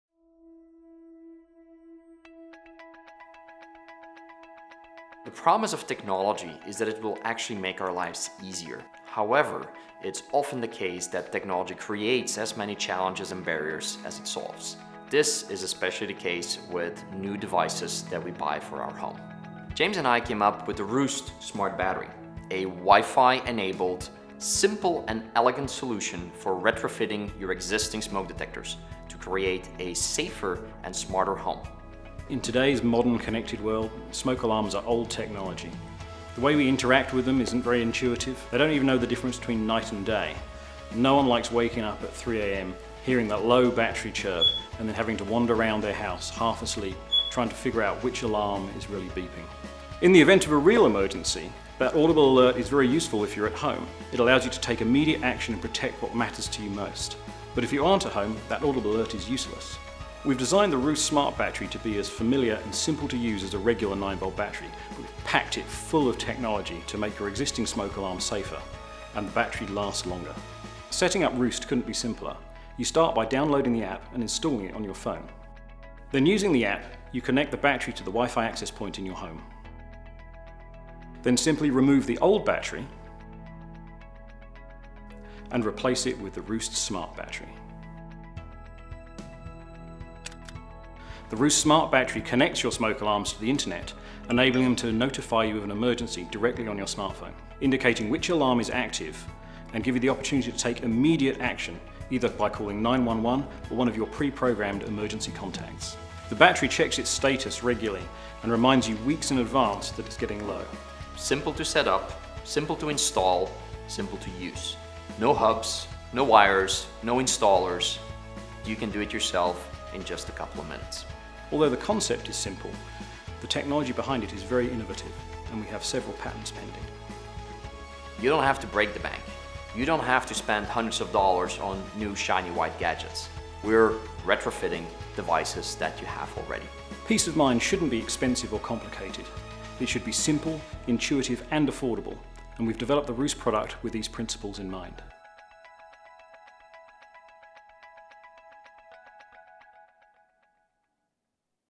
The Roost Smart Battery - Alarm notifications delivered to you, wherever you are.
Our retrofit approach to the Smart, Connected Home is different. It’s affordable - you don’t need to buy expensive new devices. In fact, you just use your existing smoke or carbon monoxide detector. It’s simple to install. No hubs, no wires and no need to hire an outside contractor. With one smart battery and an app, Roost delivers peace of mind.